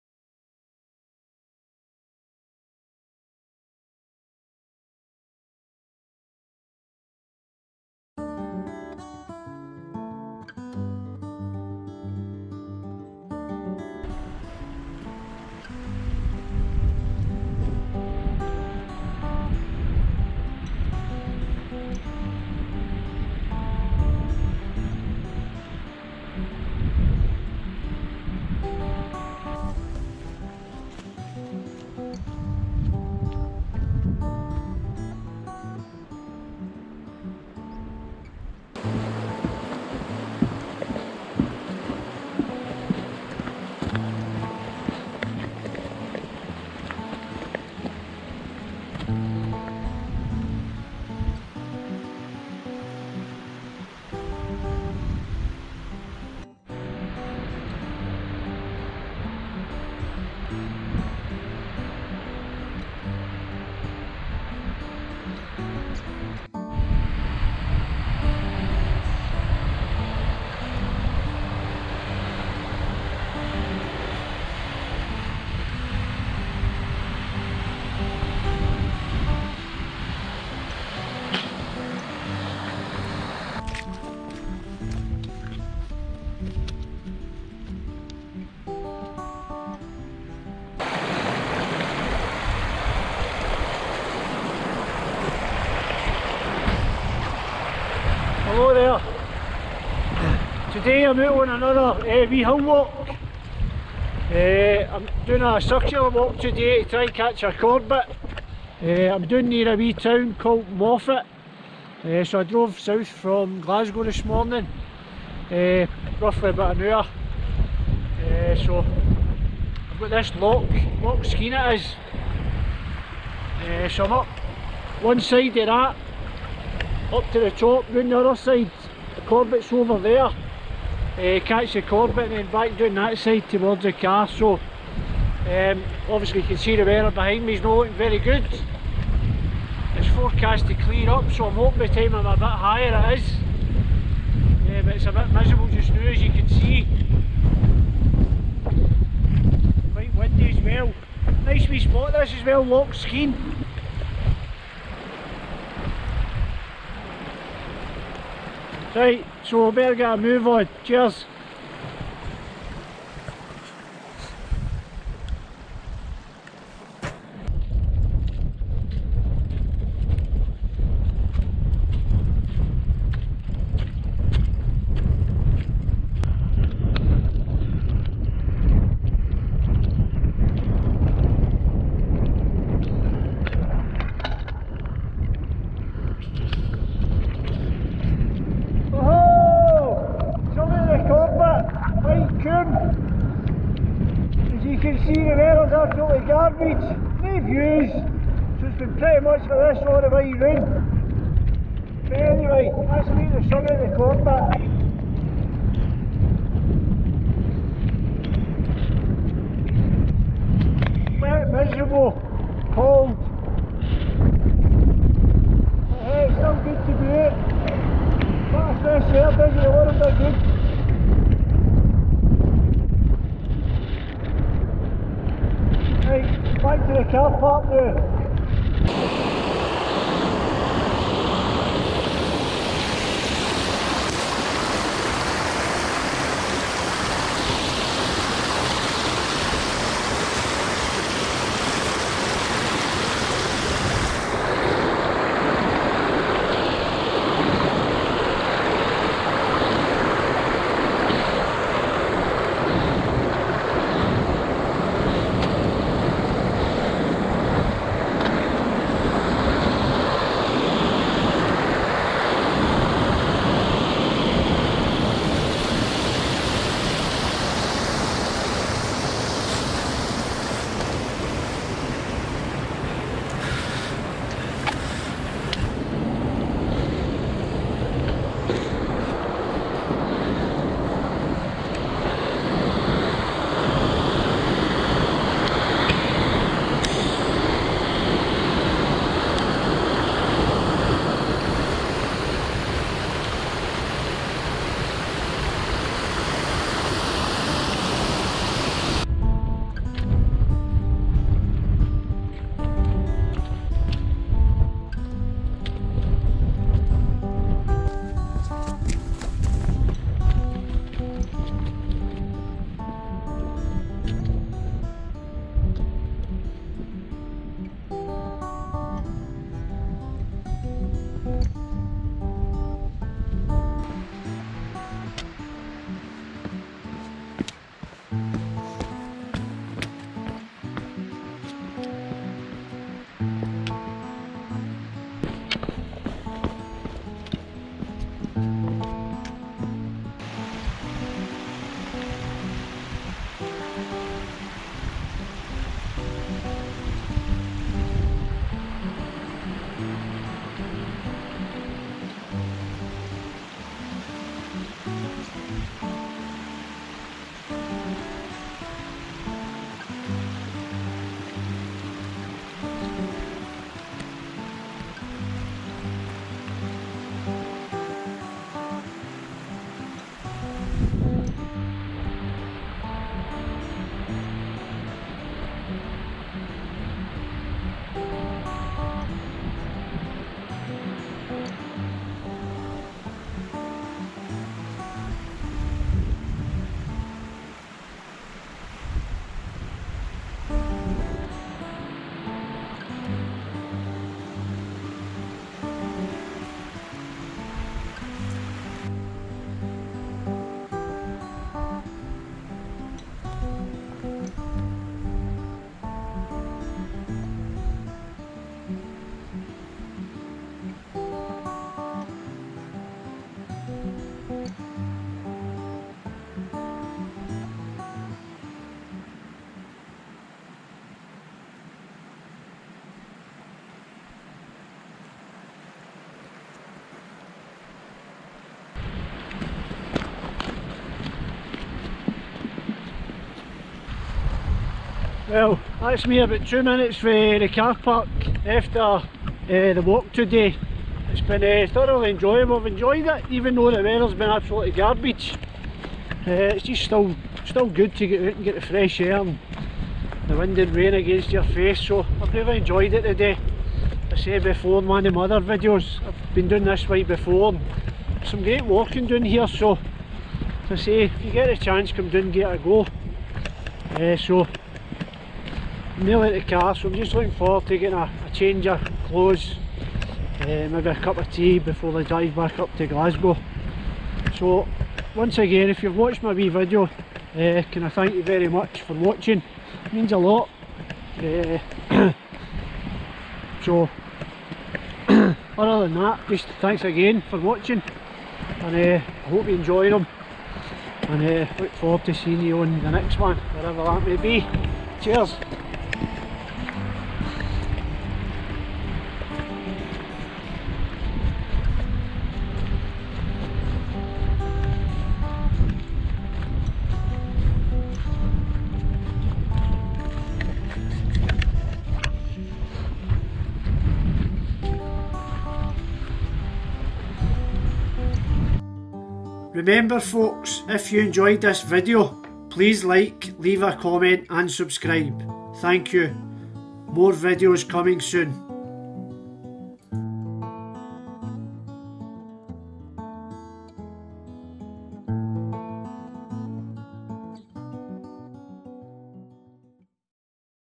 White Coomb Corbett / Grey Mare's Tail / Moffat Hills / Munros / Hillwalking / Scotland
An enjoyable circular hillwalk around Loch Skeen taking in the Corbett of White Coomb.